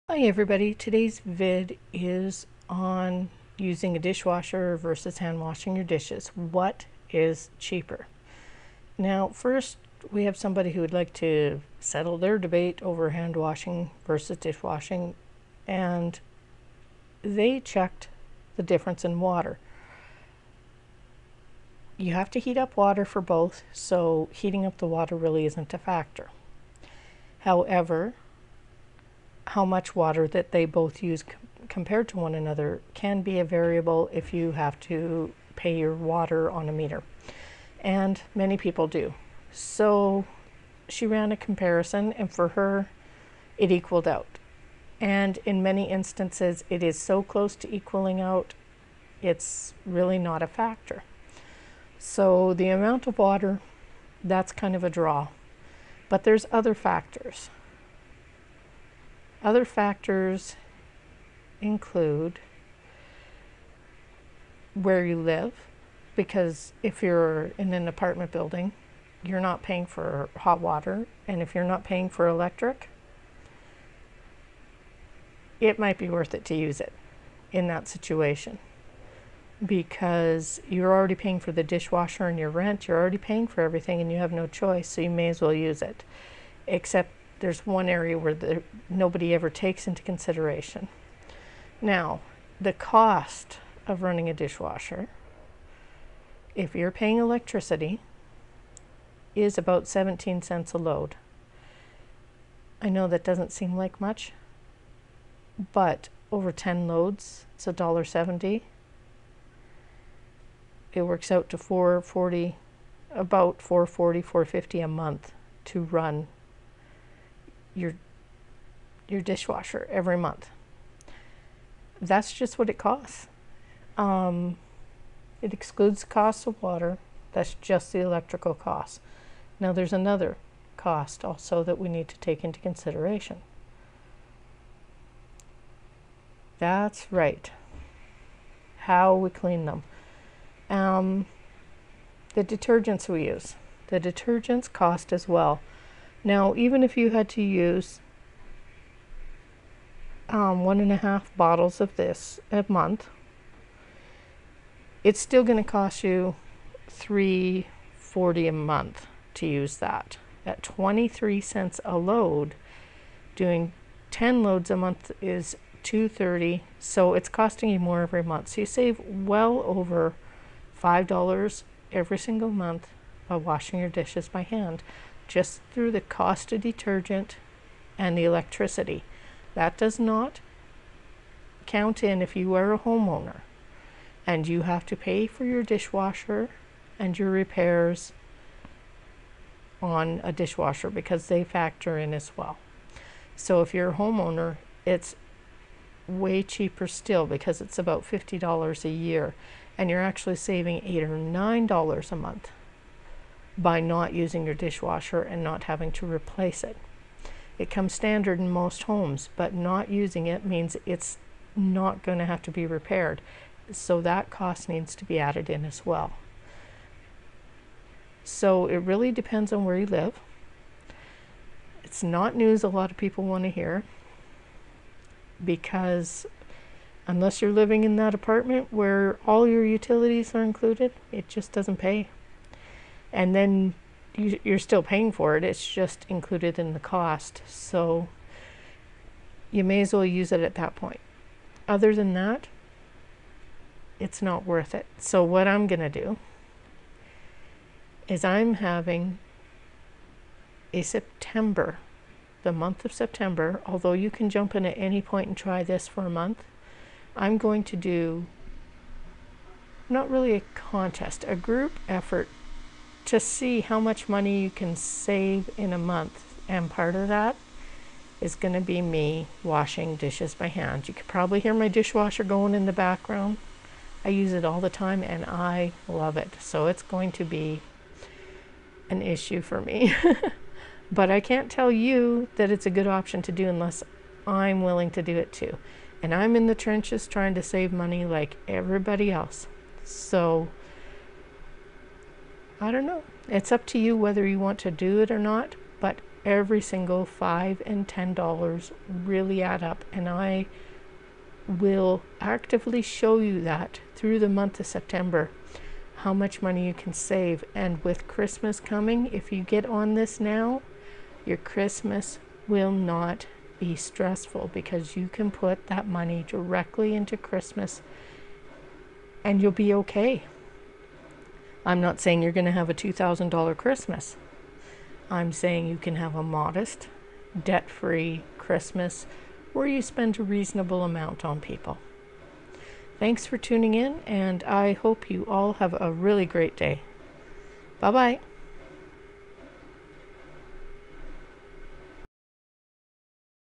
Dishwasher vs Hand washing. What is cheaper?
Most people online miss some very important variables when calculating out the costs.
I even skipped a few variables like rinse agent and glass etching over time.